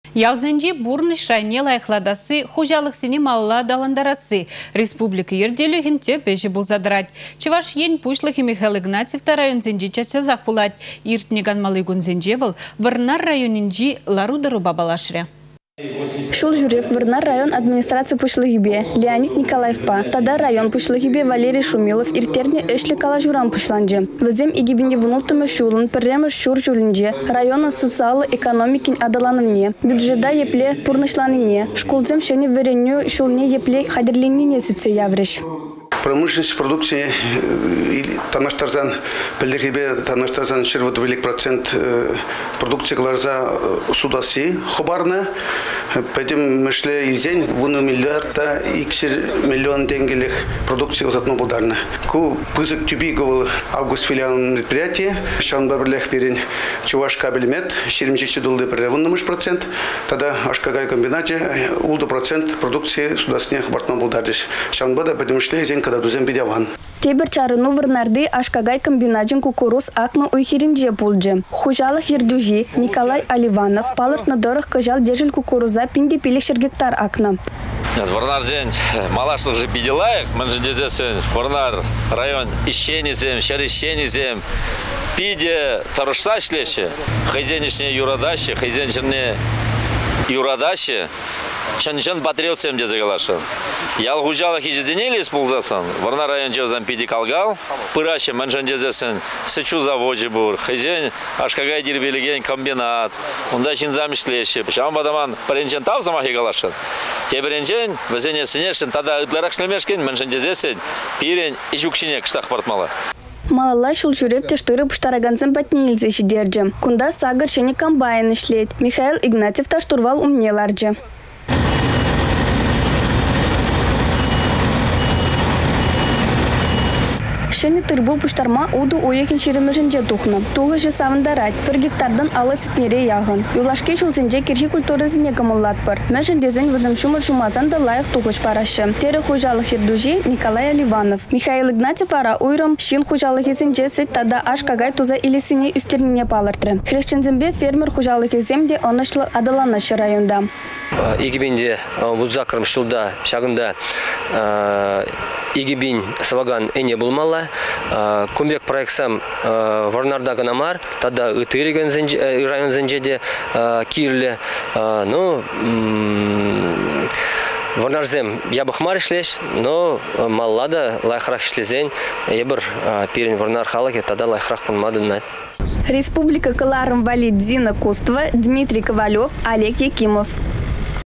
Республика Пуçлăхĕ Вăрнар районĕнче
Ялсенчи пурнăç шайне лайăхлатасси, хуçалăхсене малалла аталантарасси _ республика ертÿлĕхĕн тĕп ĕçĕ пулса тăрать. Чăваш Ен пуçлăхĕ Михаил Игнатьев та районсенче час-часах пулать. Иртнĕ канмалли кунсенче вăл Вăрнар районĕнчи лару-тăрупа паллашрĕ.